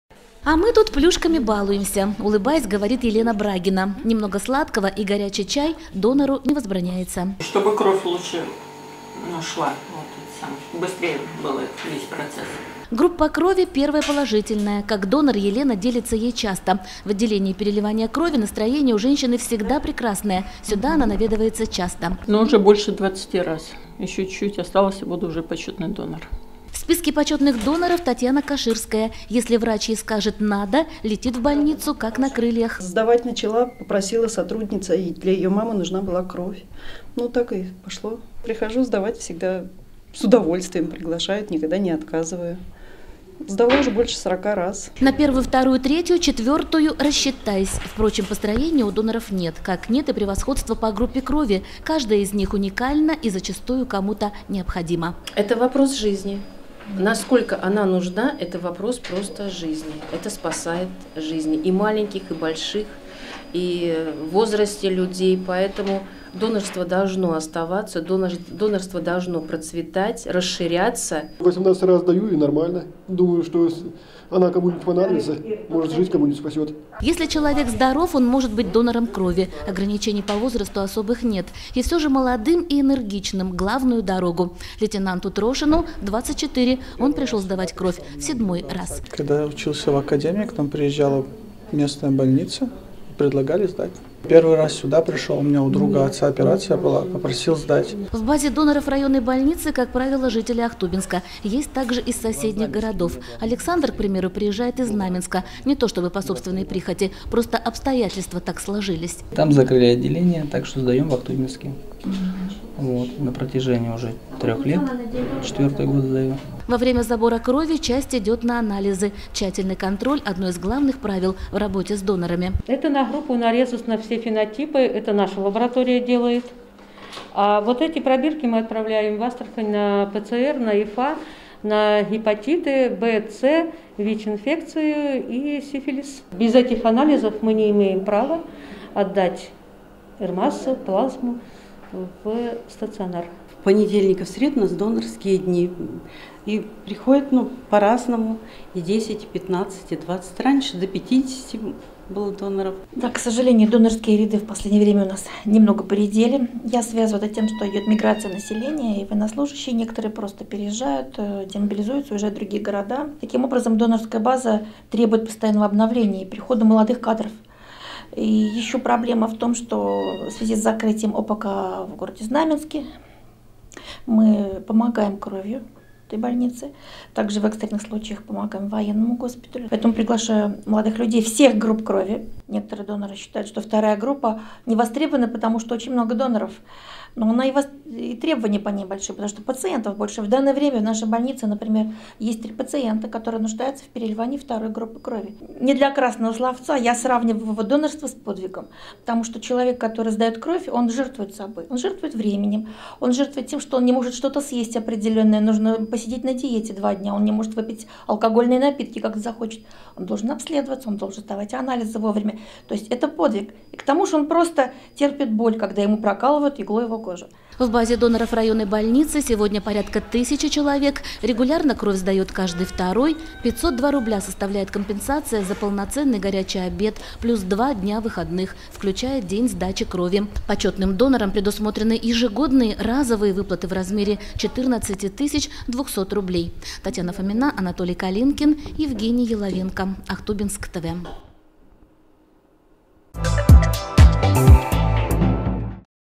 Ахтубинские доноры крови 27 03 2019
Понедельник и среда в Ахтубинской РБ - «донорские» дни.
На этой неделе кровь сдали 13 доноров.